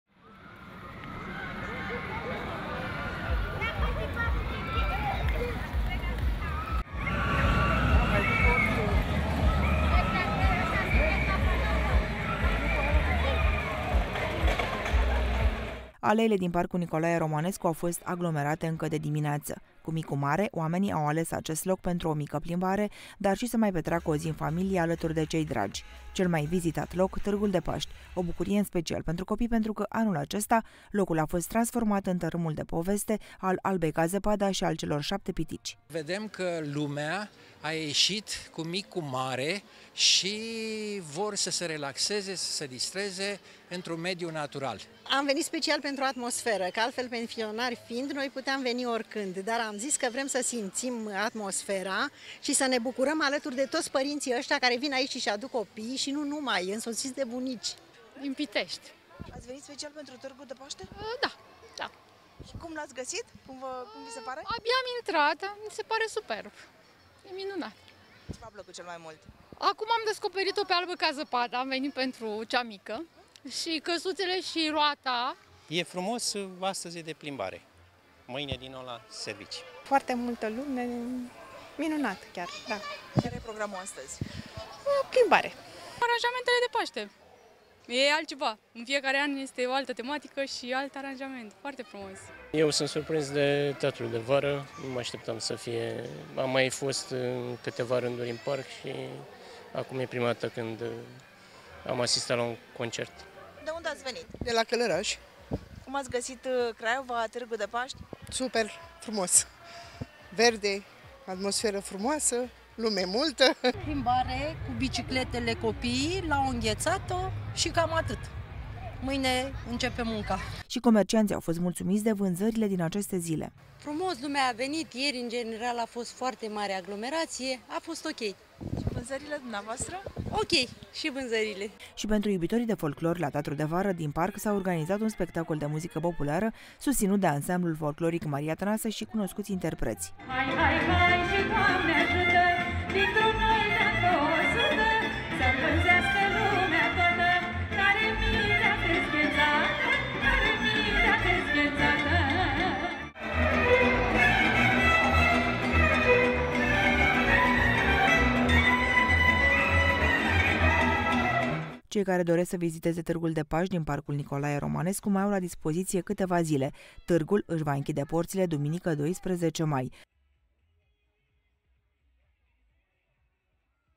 TÂRGUL DE PAŞTE, DESTINAŢIE PREFERATĂ DE TURIŞTI
Târgul de Paşte din Parcul ,,Nicolae Romanescu” din Craiova a fost locul preferat de craioveni şi, în special de turişti, astăzi, în a doua zi de Paşte. Sute de oameni au venit special să petreacă câteva ore în natură şi să se bucure de atmosfera de poveste de la Târg. Toţi au dorit să profite de vremea frumoasă şi de ultima zi liberă, iar Târgul de Paşte a fost locul perfect.